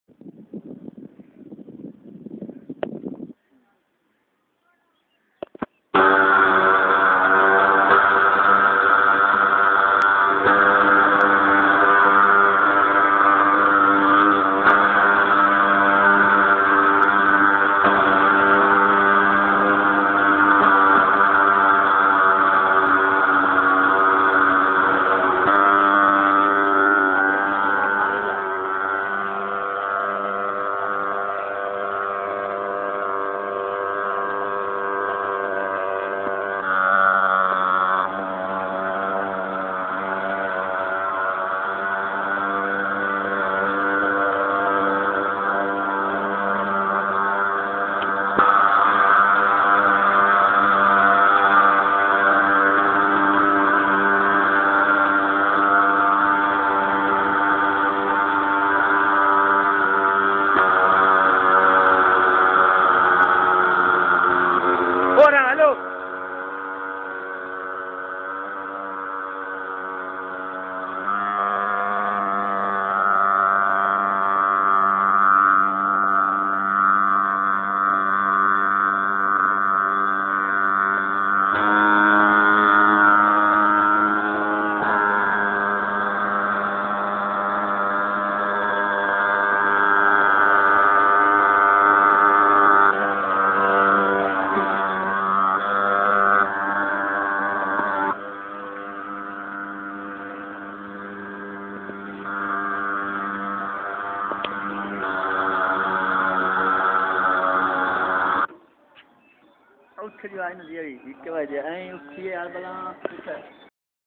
Dengue Spray at Govt. High School Pakkay Wala Jhang
This is the video of dengue spray which was done to save students and children form dengue on the orders of School Education Department Govt, Of the Punjab.